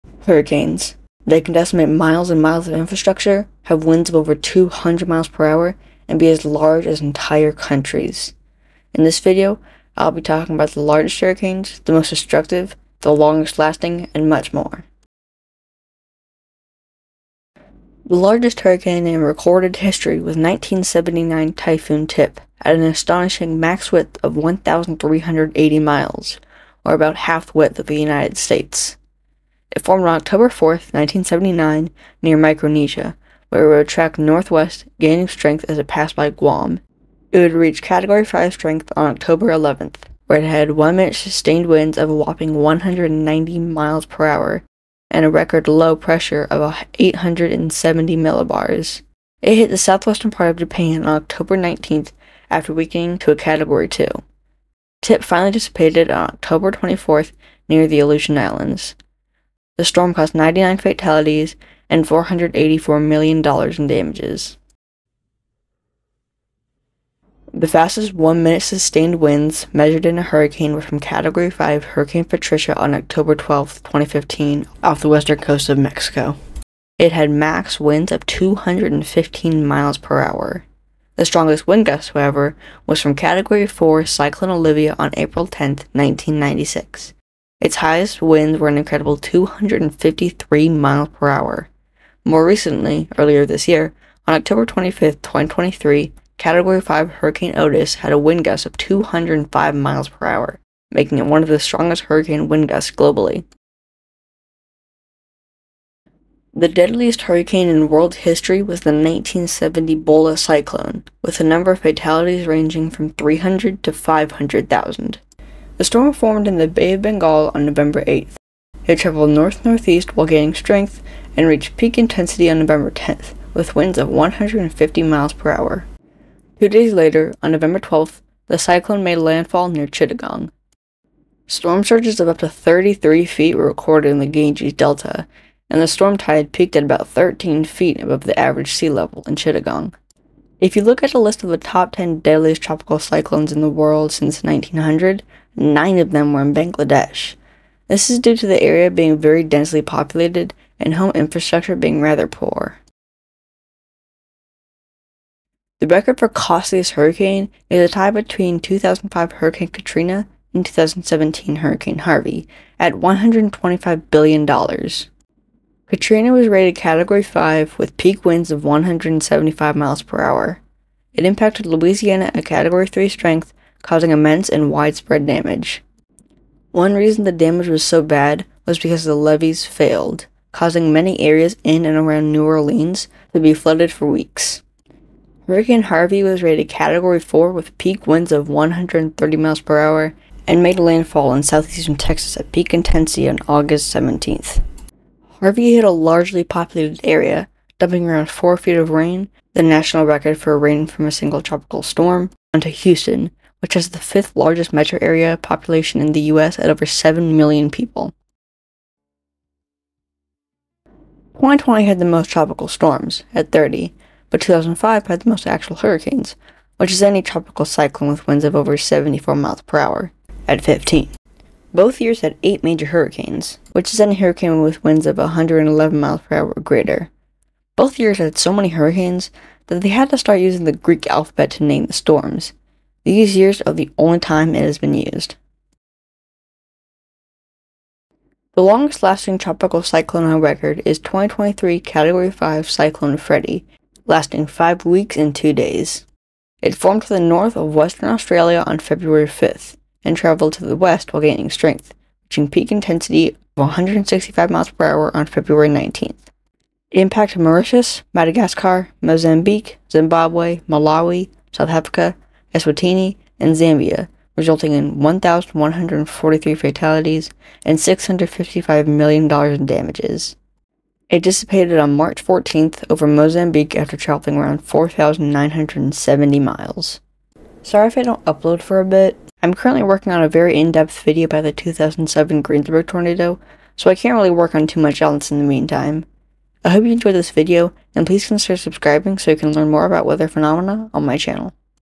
Hurricane Records | Fastest, Costliest, Largest, and more
Hurricanes. They can decimate miles and miles of infrastructure, have winds of over 200 Mph, and be as large as entire countries. In this video, I'll be talking about the largest hurricanes, the most destructive, the longest lasting, and much more.

0:00 Intro
0:15 Largest
1:05 Fastest Winds
1:44 Deadliest
2:33 Costliest
3:29 Most in a year
3:56 Longest Lasting
4:37 Outro